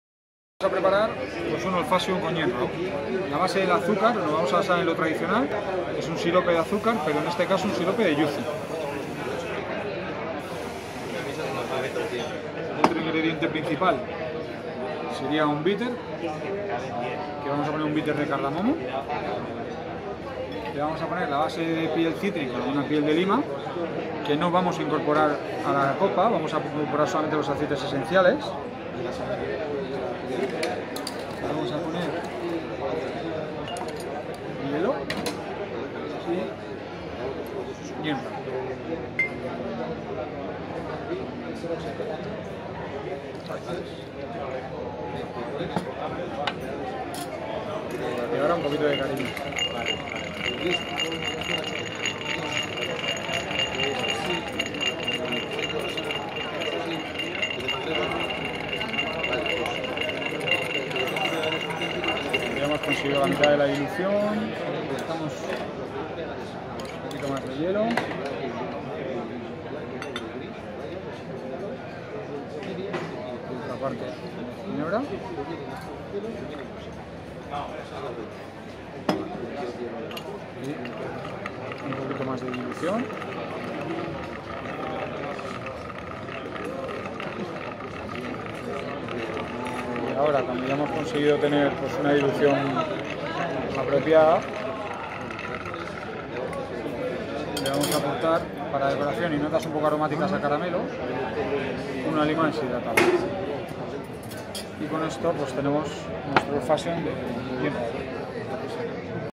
Old Fashioned con ginebra GinRaw
El bartender Luismi García prepara una adaptación del clásico Old Fashioned utiilizando ginebra en vez de whiskey. Este cóctel lleva ginebra GinRaw, sirope de yuzu, bitter de cardamomo y piel de lima.